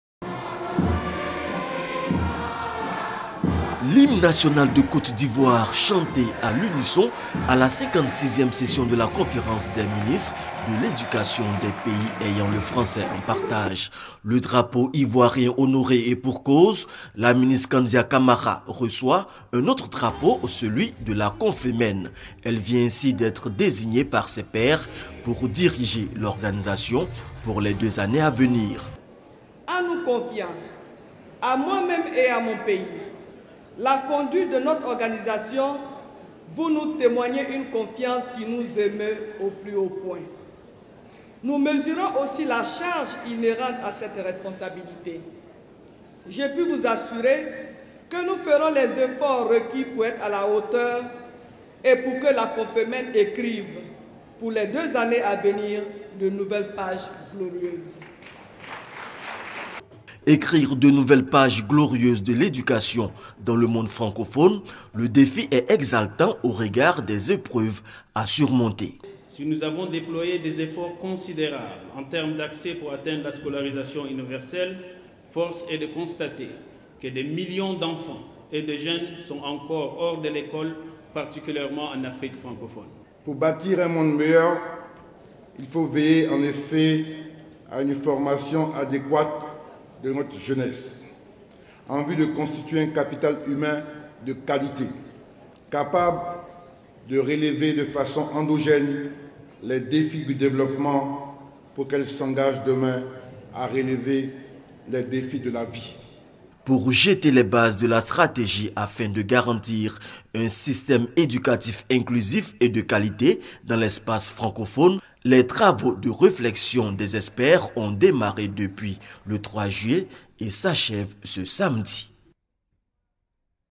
CONFEMEN: le Ministre Hamed BAKAYOKO à la 56è Conférence
Jeudi 10 juillet 2014. Hôtel Ivoire Sofitel, Abidjan. La 56è session ministérielle de la Conférence des ministres de l'éducation des Etats et gouvernements membres de la Francophonie (CONFEMEN) s`est ouverte, autour du thème « éducation inclusive et de qualité pour tous en Francophonie : défis, priorités et perspectives pour l'après 2015».